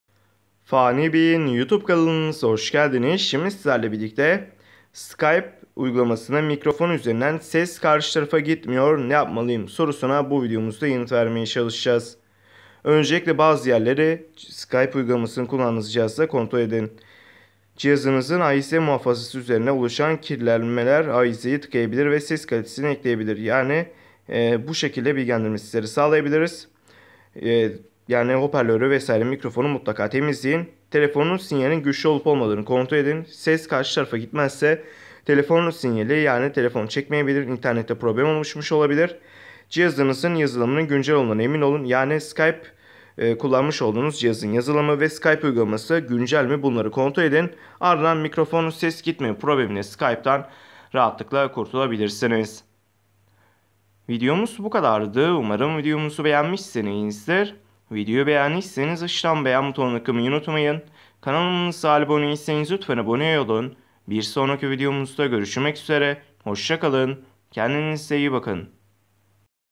Skype Mikrofon Ses Gitmiyor Sorunu Çözümü
Merhabalar bu videomuzda sizlerle birlikte skype mikrofon ses gitmeme sorunu çözümü nasıl yapılır sorusuna yanıt vermeye çalışıyoruz.İyi Seyirler Dilerim.

skype mikrofon nasıl açılır
skype mikrofon ses sorunu nasıl düzelir
skype mikrofondan ses gitmiyor